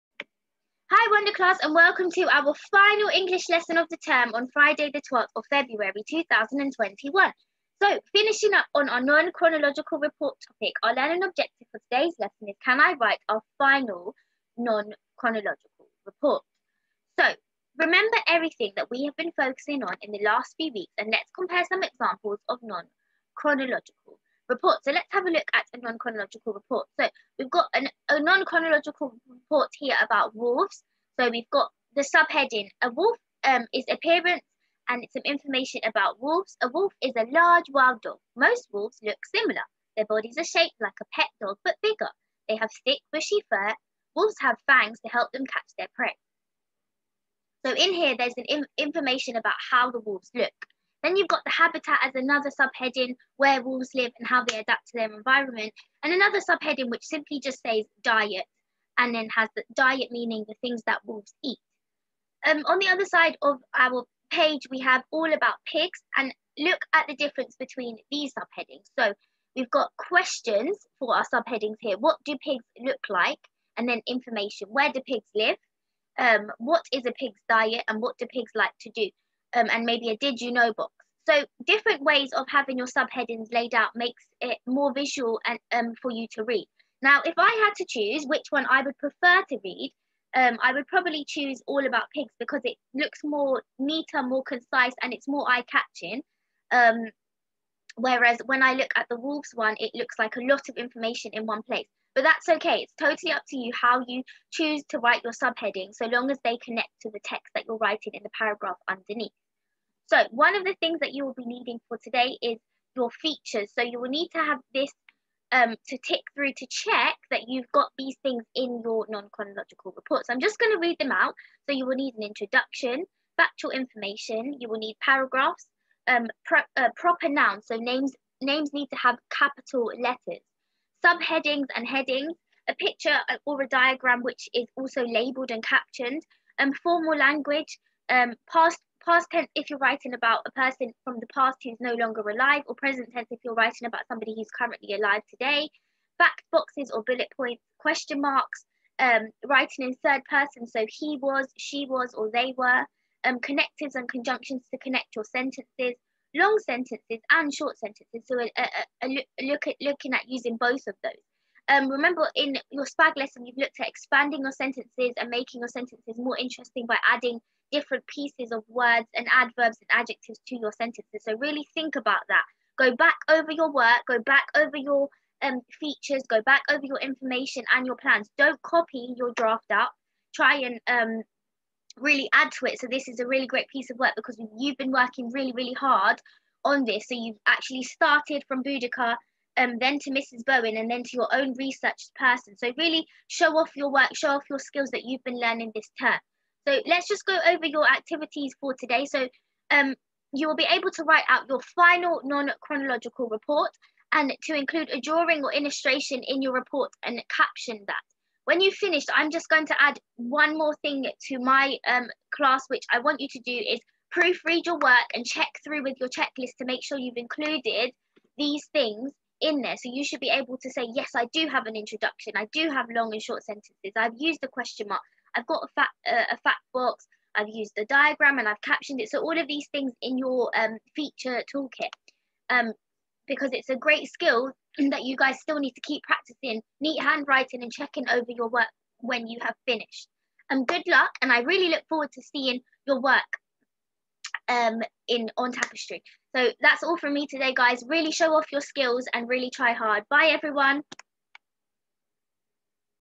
Year 4 12th February English Wonder
In today's lesson, can you write your final non-chronological report? Please upload any work onto Tapestry by 4pm today.